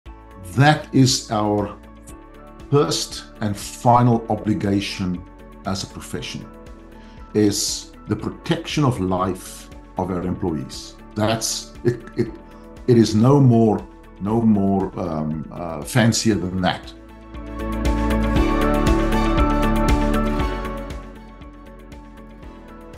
‘THIS’ is our FIRST & FINAL obligation as a professional
Safety on the Edge: Pioneering the Future of Safety Practices

Tune in to hear thought-provoking discussions and gain valuable insights into the future of safety practices from some of the leading voices in the field.